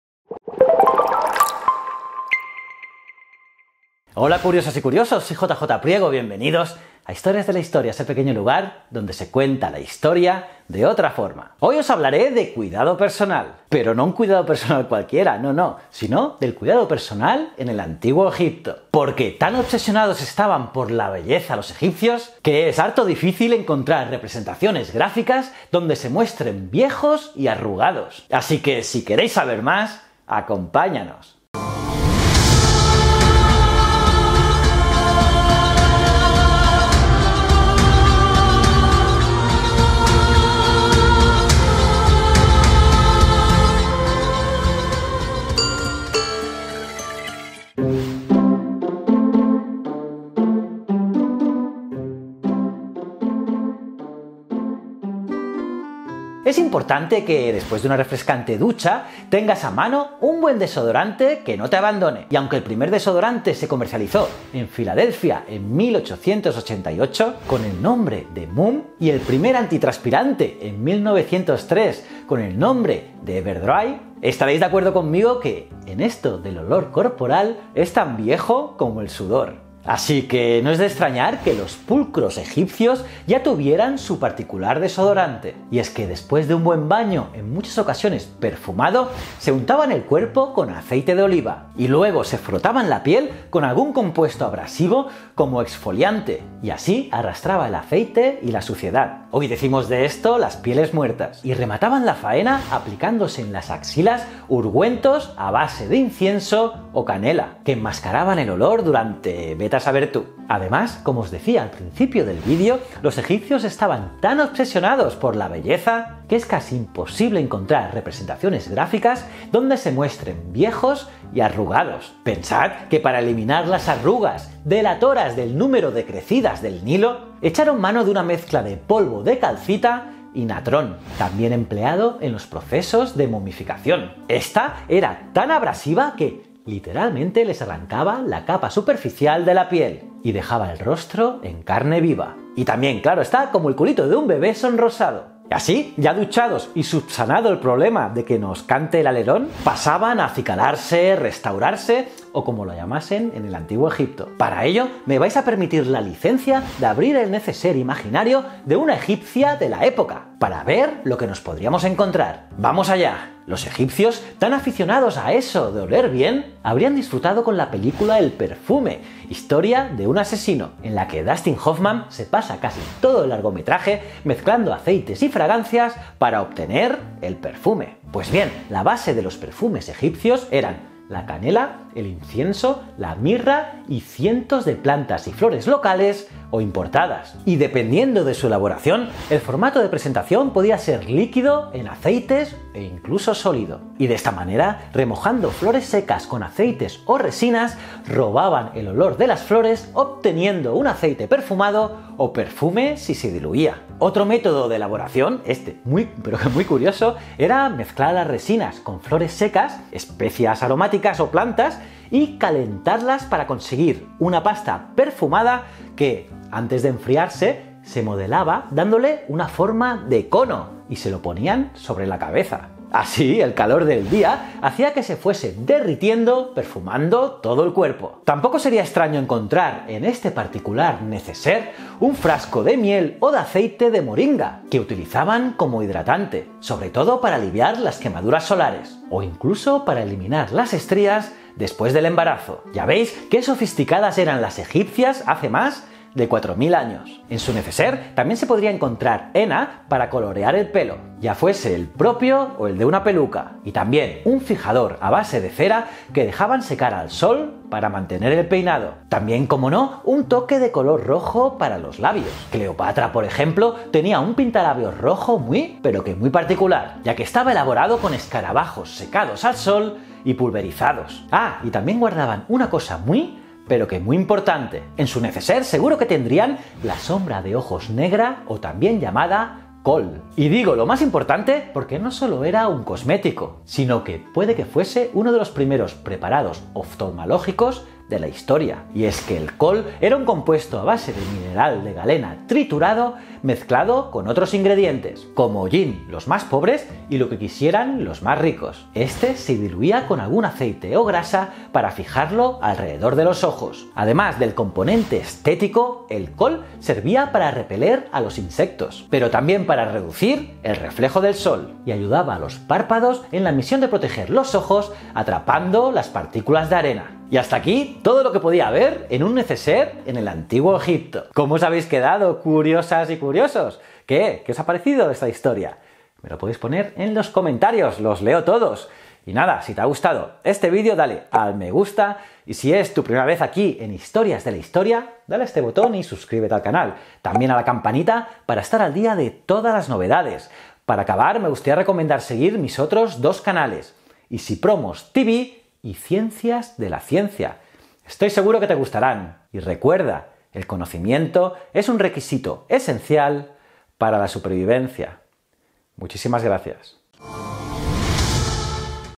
👁️ ¿Qué encontraríamos en el NECESER de una MUJER en el ANTIGUO EGIPTO? 🤔
Hoy os hablaré, de cuidado personal… pero no un cuidado personal cualquiera. Sino, del cuidado personal en el Antiguo Egipto…

Porque, tan obsesionados estaban, por la belleza los egipcios, que es harto difícil encontrar, representaciones gráficas, donde se muestren viejos y arrugados.

𝓜𝓾𝓬𝓱𝓪𝓼 𝓰𝓻𝓪𝓬𝓲𝓪𝓼

𝓙𝓙 😘👌